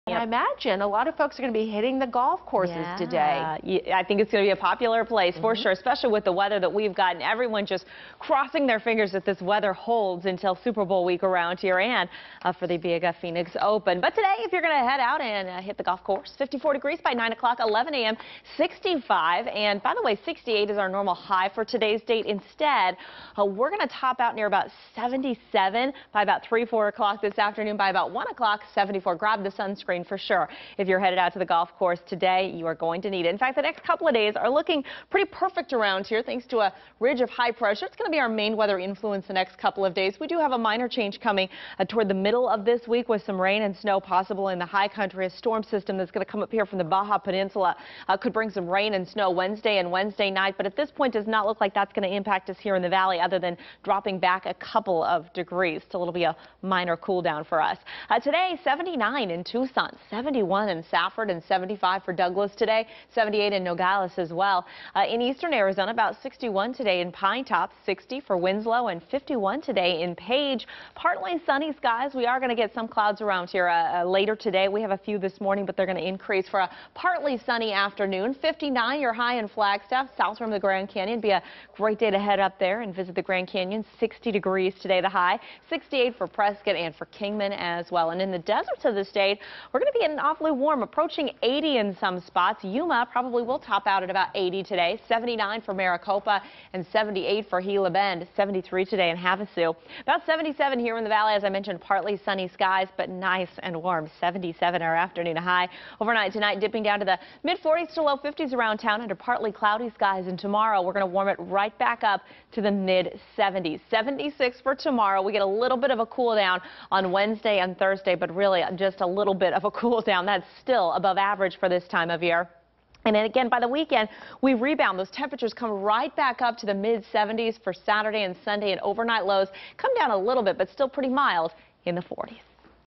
Warm weather continues
A partly sunny and warm Monday is forecast for the Valley, with an afternoon high temperature that's expected to top out near 77 degrees.